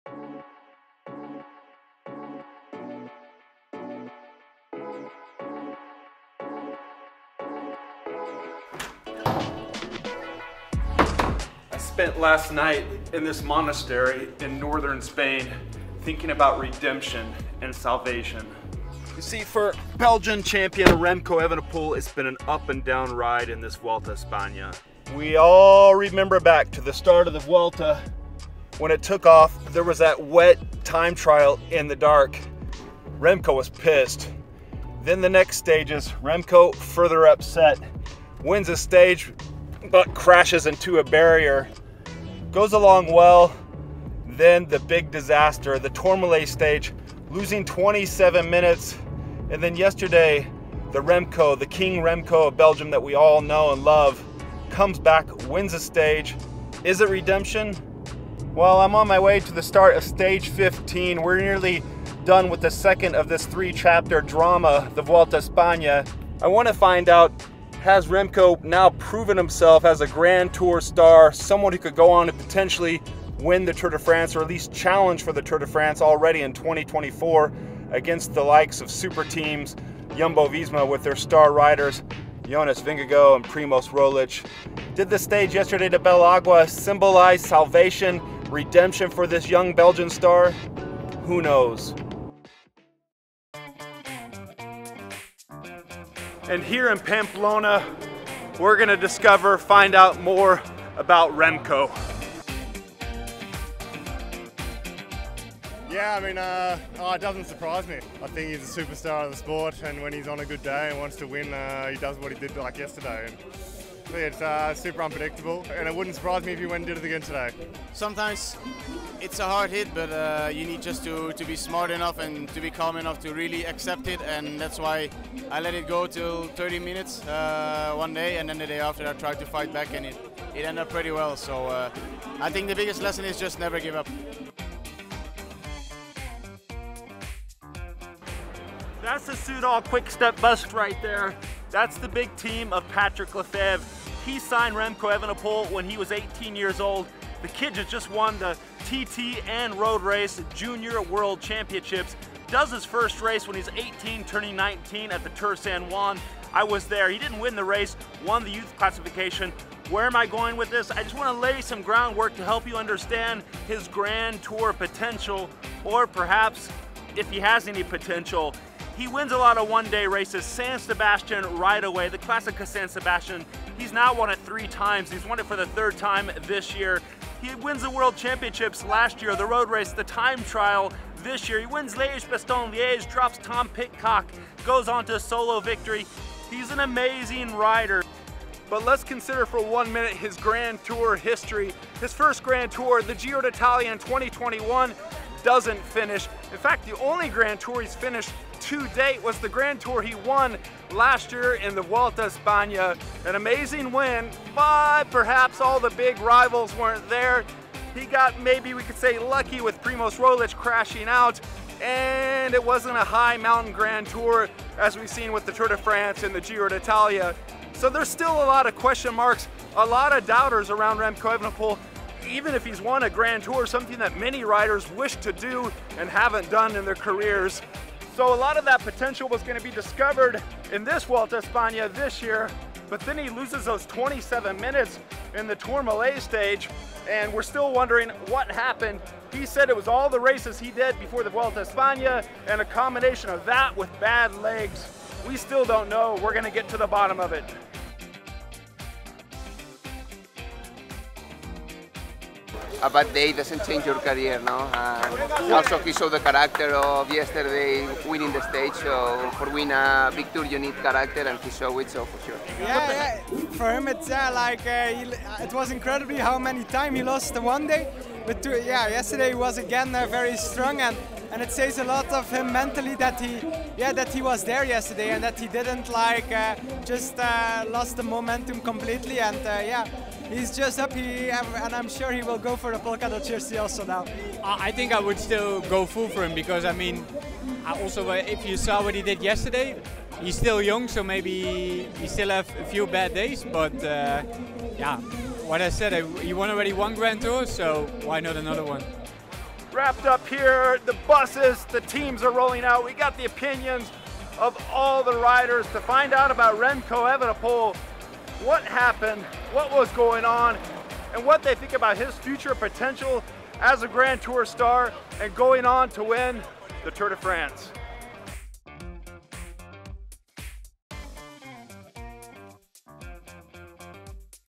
Remco Evenepoel The Grand Tour Real Deal For Tour de France? | Chasing The Pros
Is Remco Evenepoel the real deal in grand tours with eyes on the Tour de France 2024? After losing 27 minutes in the Vuelta a España, Gregor Brown talks to the pros to find out.

Presented by @Hammerhead